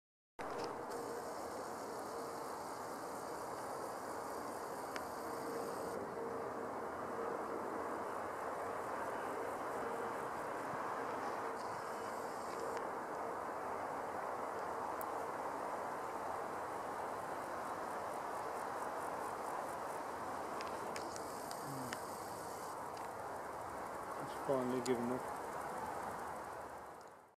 Large White mating failure
She wants to, but he can't. ?

Sutton Park, 30th July 2013.